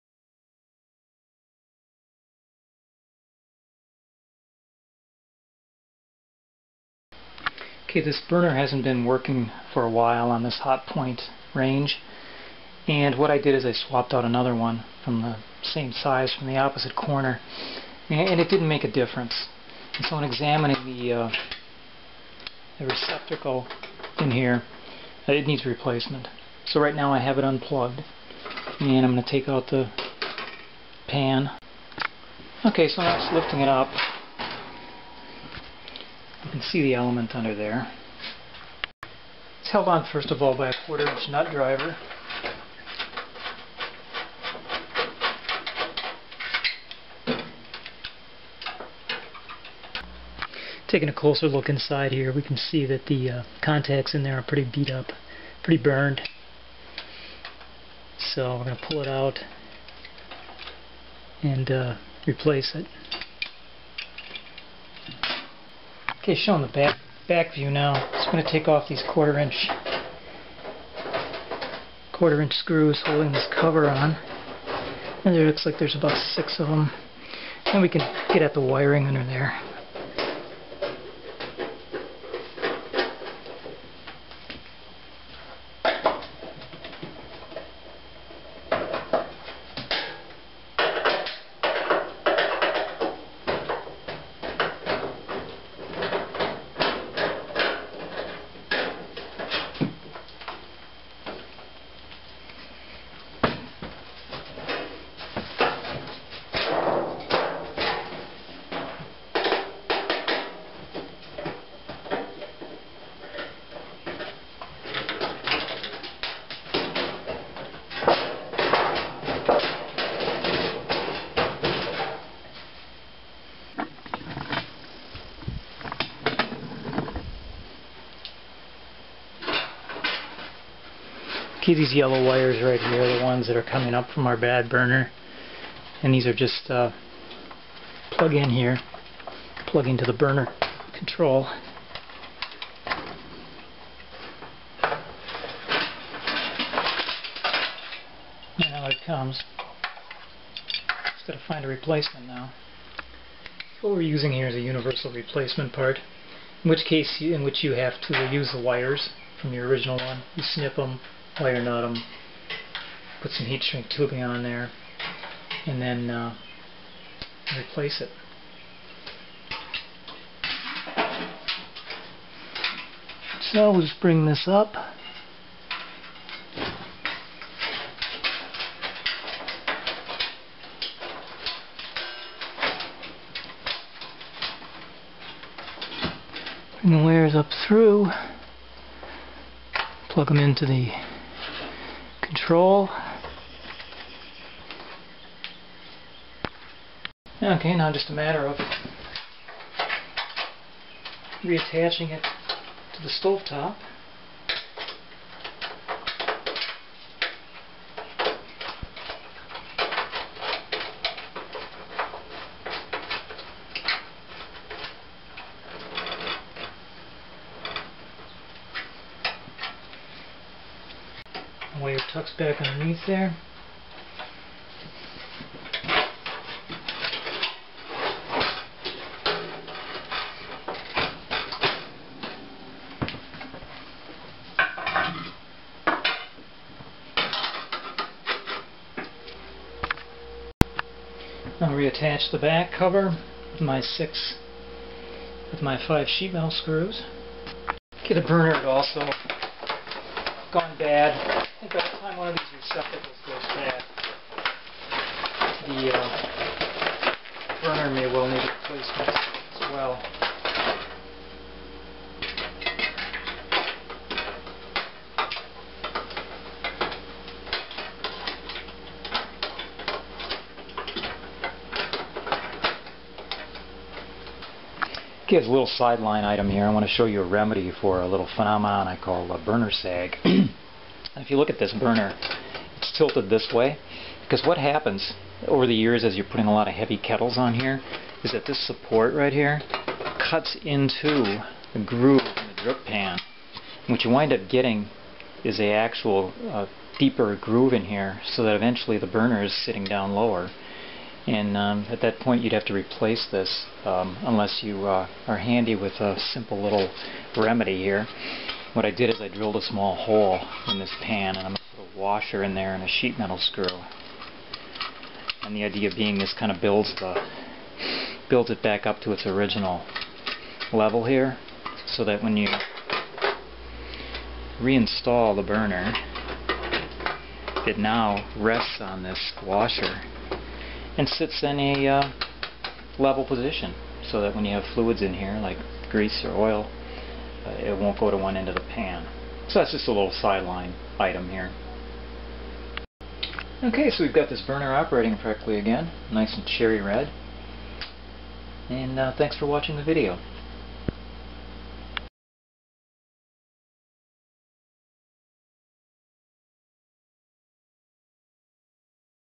How to replace a burner receptacle on a Hotpoint range
Some handy tips on how to replace a bad burner receptacle on Hotpoint range model RB757B B1WH. Includes bonus help with restoring a worn drip pan that results in a slanted burner mount. For text and still pictures of this procedure